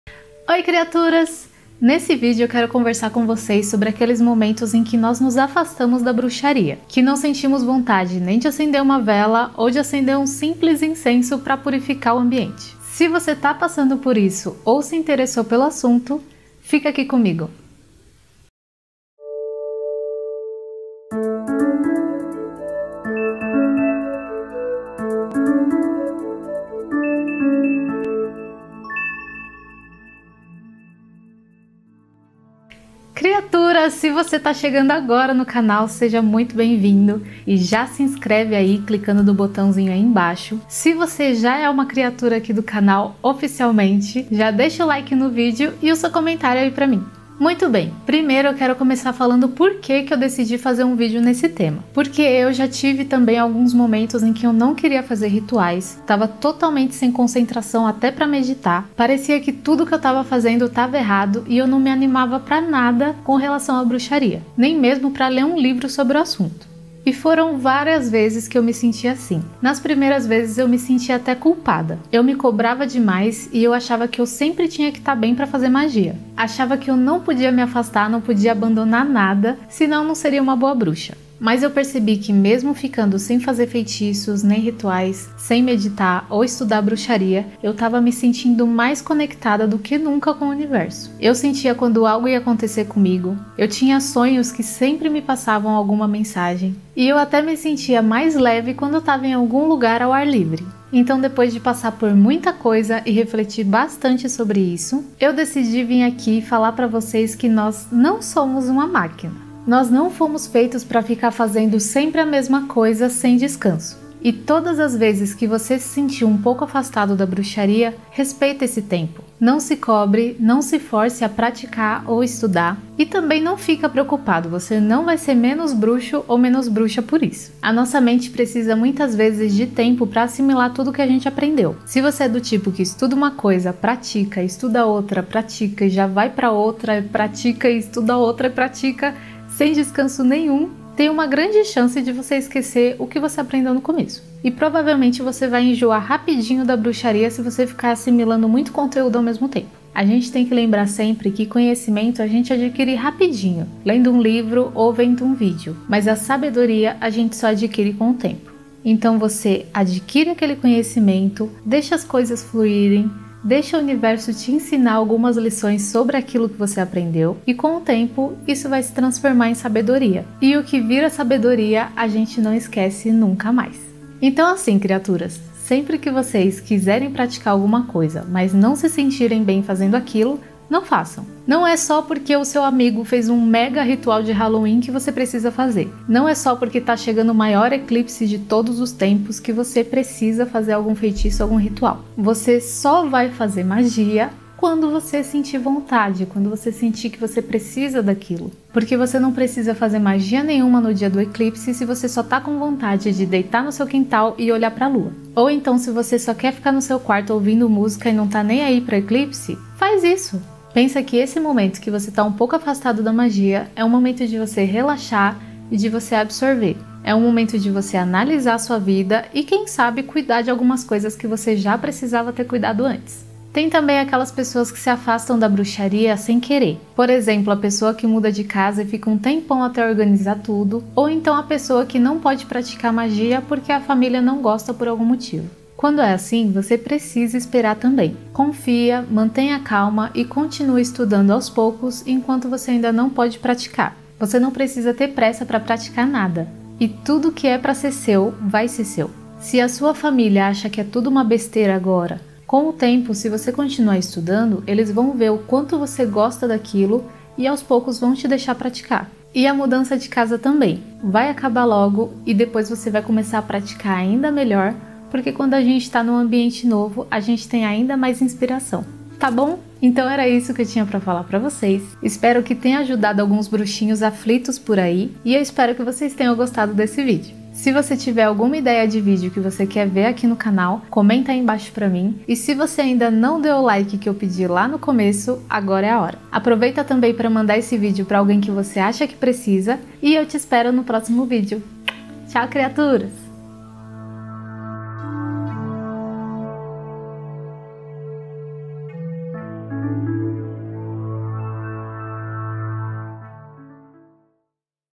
Me afastei da Bruxaria
Esse vídeo foi feito para aquelas bruxas e bruxos que se sentem mal por não terem vontade de praticar bruxaria em algumas fases da sua vida. Um dia todos vamos passar por isso, mas a gente não deve entrar em desespero. Keep calm.. e só keep calm mesmo 😅💜